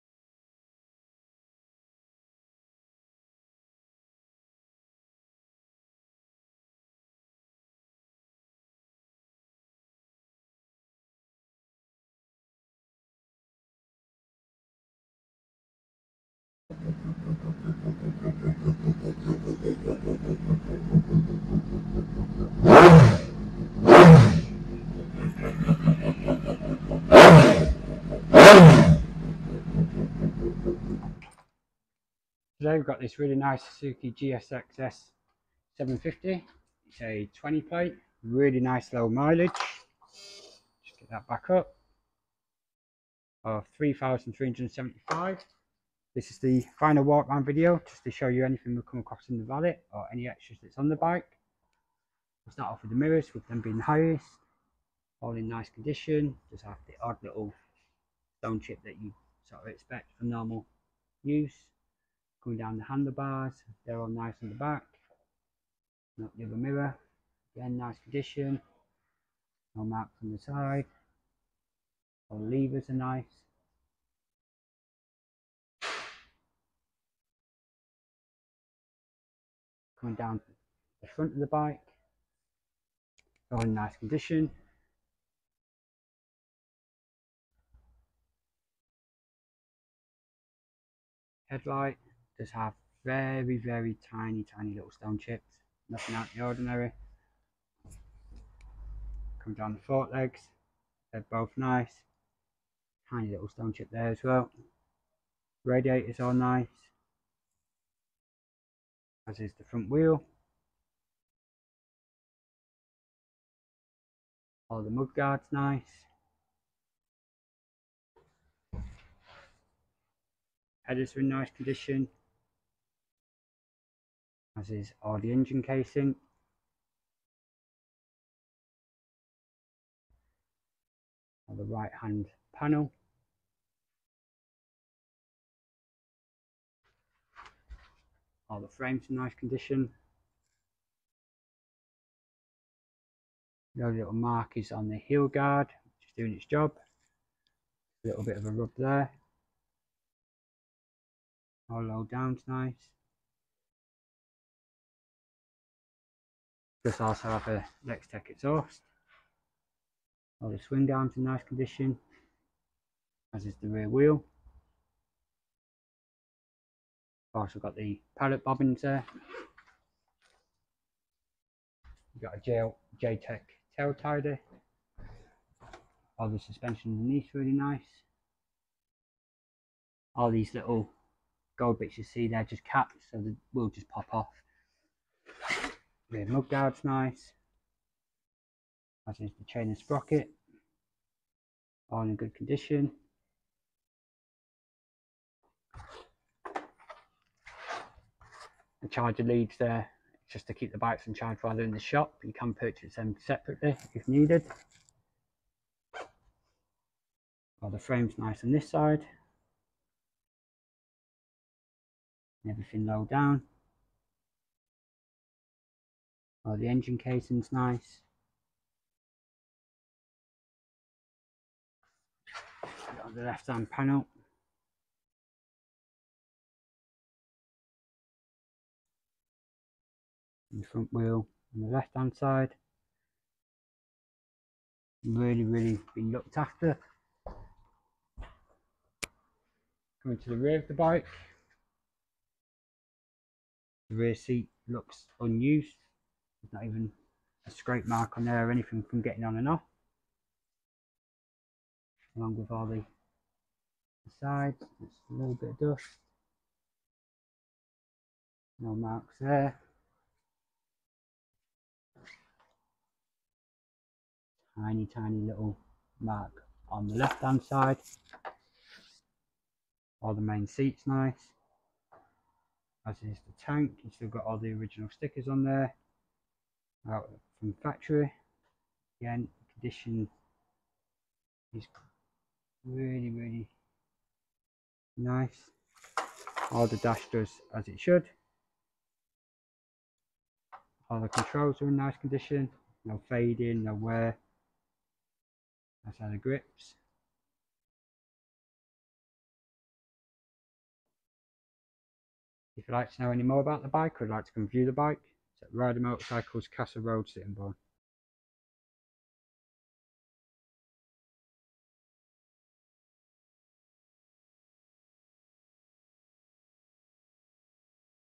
2020 Suzuki GSXS750 - Used Bike Video Tour
We have this 2020 Suzuki GSXS750 in stock now. Come and view it for yourself in store or buy